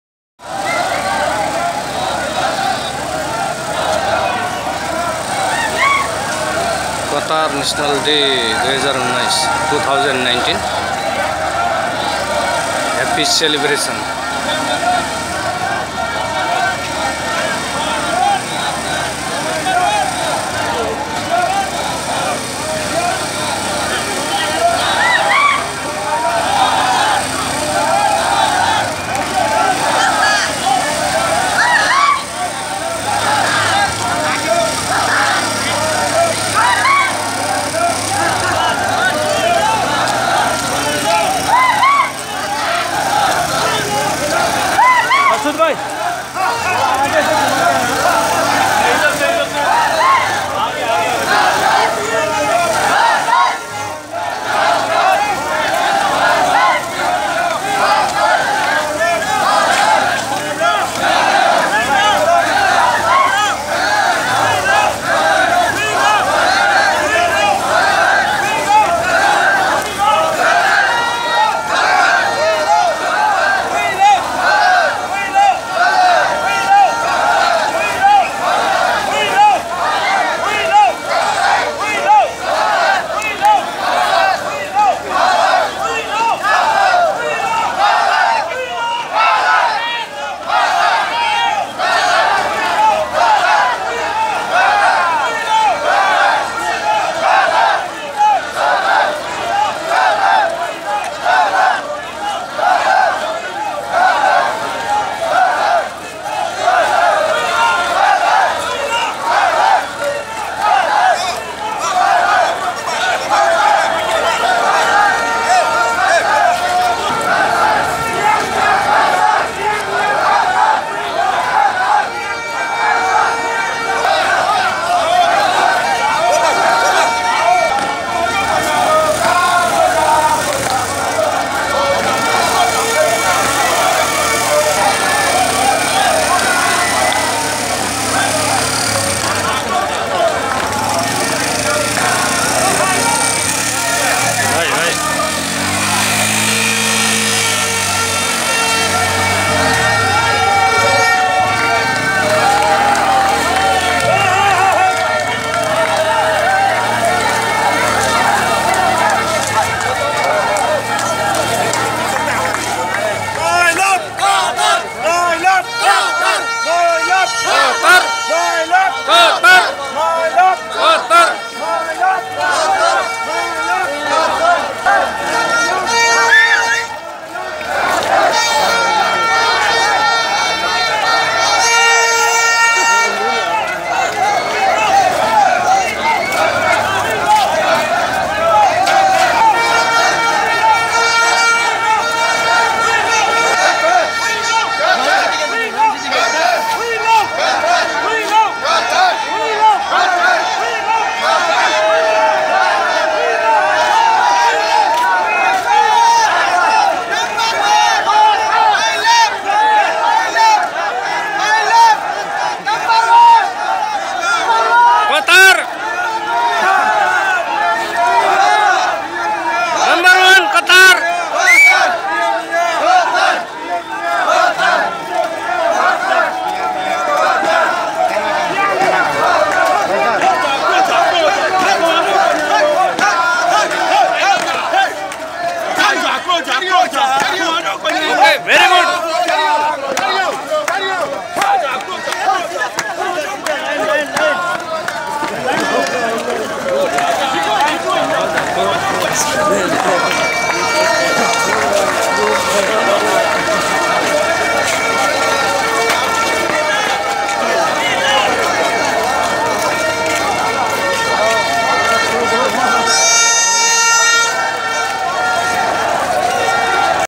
Qatar national day # happy celebration # Labor City Asain Town
कतार नेशनल डे को अवसरमा आज कतारको श्रमजीवी क्याम्पमा अत्यन्तै भव्य र सभ्यताको साथमा यसरी खुशियाली मनाइयो ।